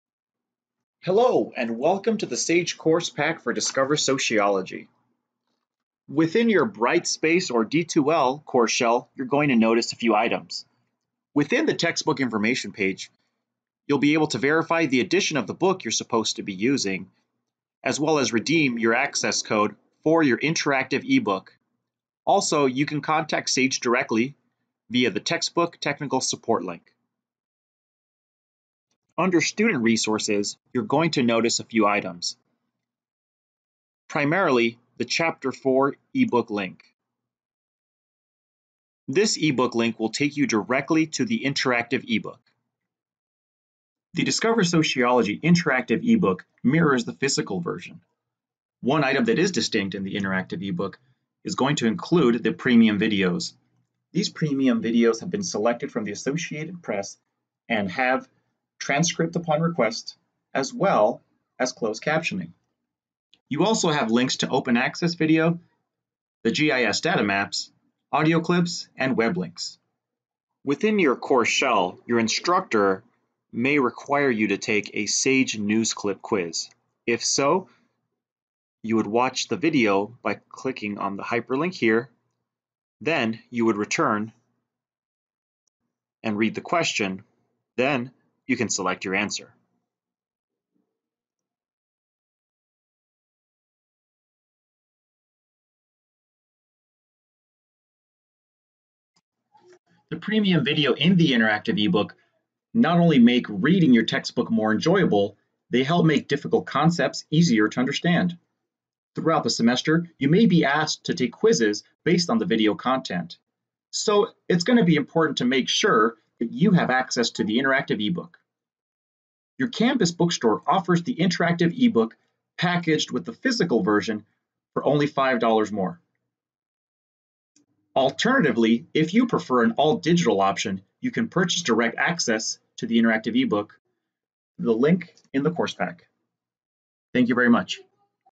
Discover Sociology D2L- FDOC
This is the First Day of Class video for Chambliss Discover Sociology in D2L